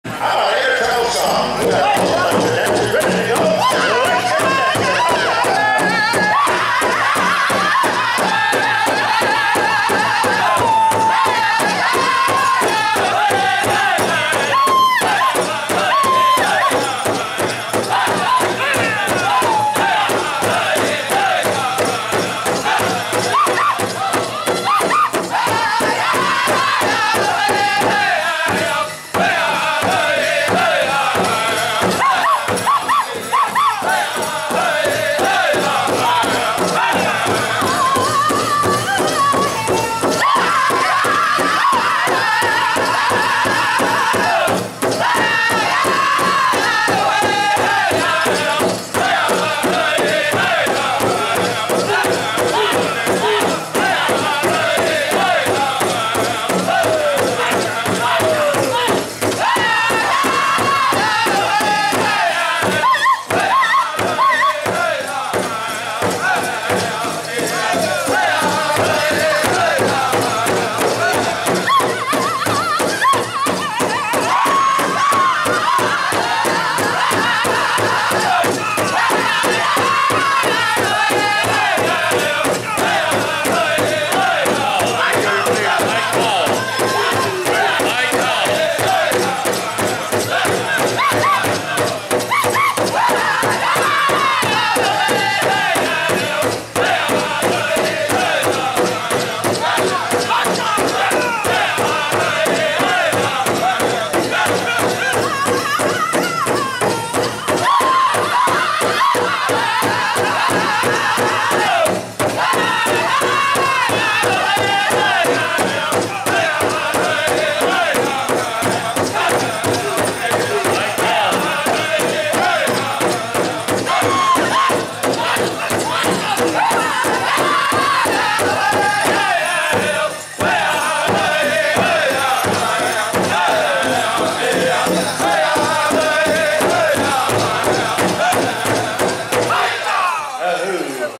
Ho-Chunk Station - FNL Song 1 in Mandaree July 14, 2017
Friday Night Lights Intertribal Contest Singing Round 1 at Mandaree Celebration 2017 July 14 - 16, 2017 in Mandaree, North Dakota